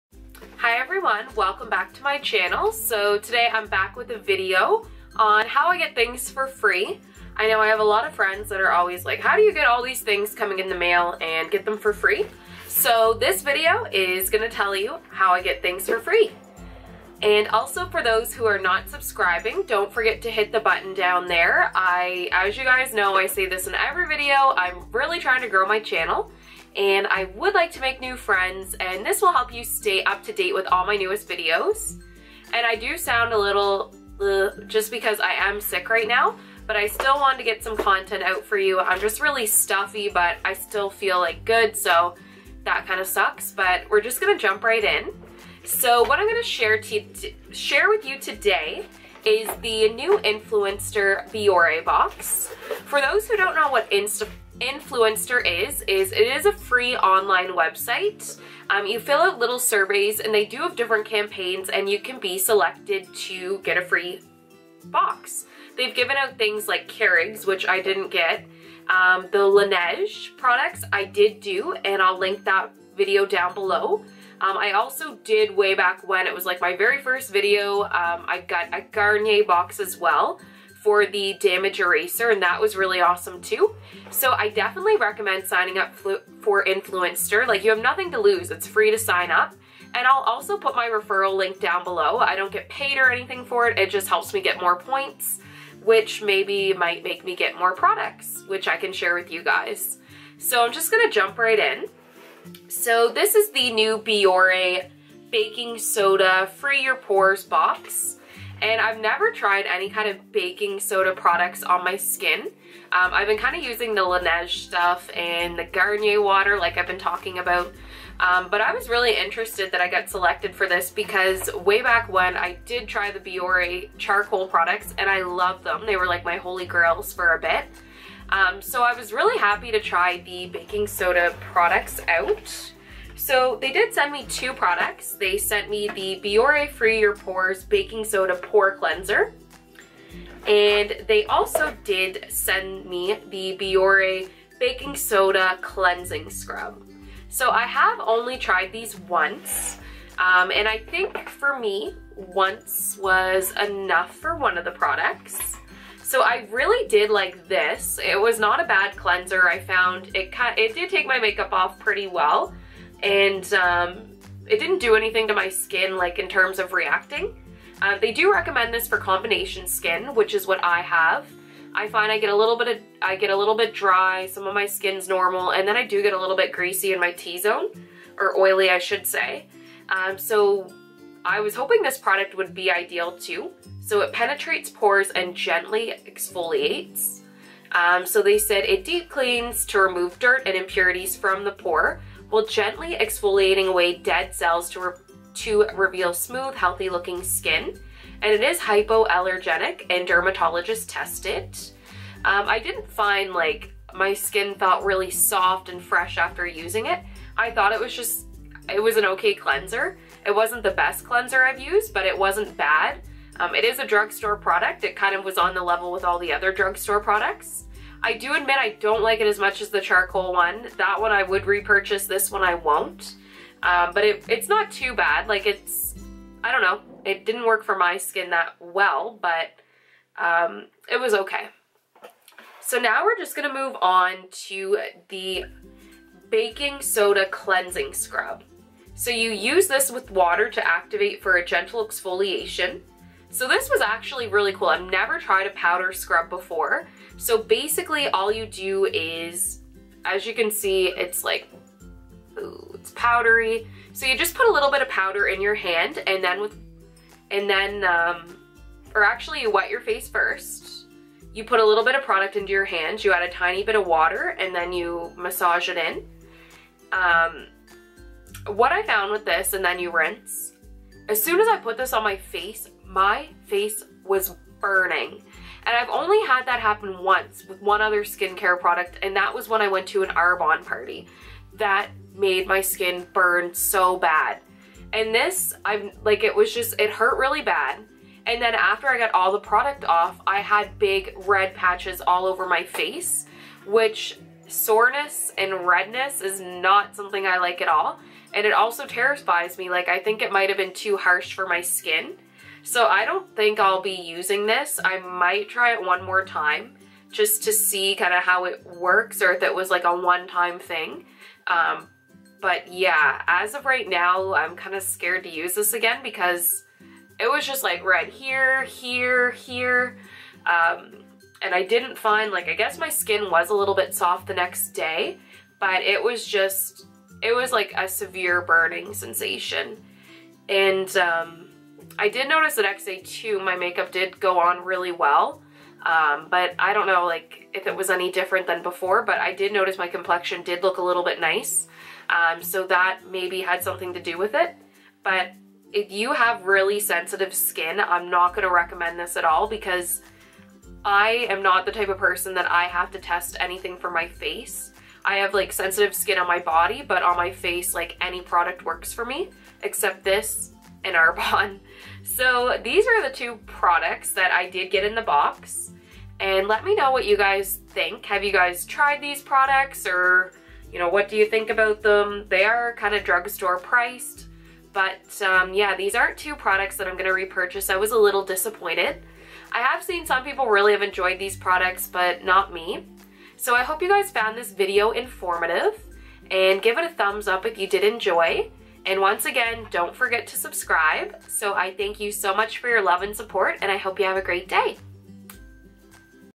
How I Get Things For Free| Influenster Biore Baking Soda Unboxing
Hi lovelies,
Today I am back with a new influenster unboxing. This is for the new Biore Baking Soda products. I was happy to be chosen to try these two new products. I received the Biore Baking Soda Cleansing Scrub and Biore Baking Soda Pore  Cleanser.
If you are interested in joining Influenster here is my

XOXO,
Jasmine

Save 20% at Newport Skinny T using the code Jasmine20

I received these products for free for testing purposes.